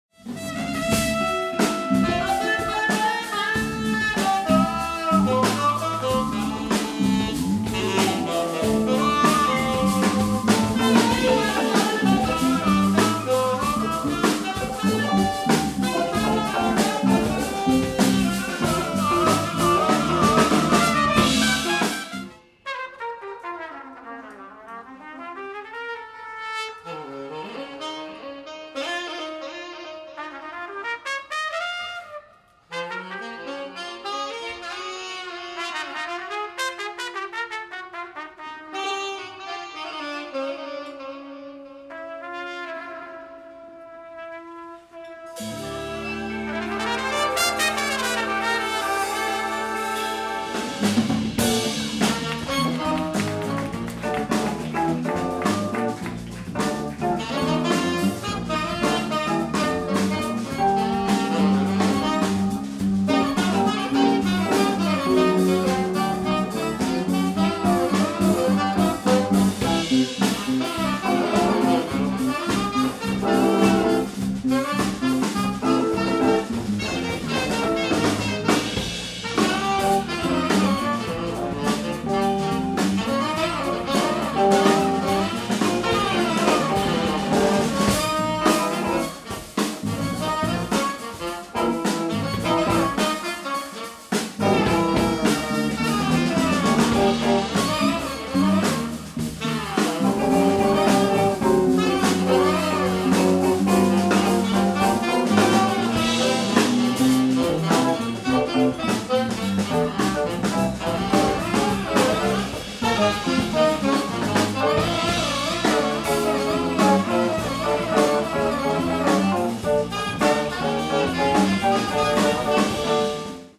Applying FX Effect on my Tenor Saxophone Solo
Basic Notes Jazz Combo

Venue: Lost Stars Livehouse Bar & Eatery, Tai Kok Tsui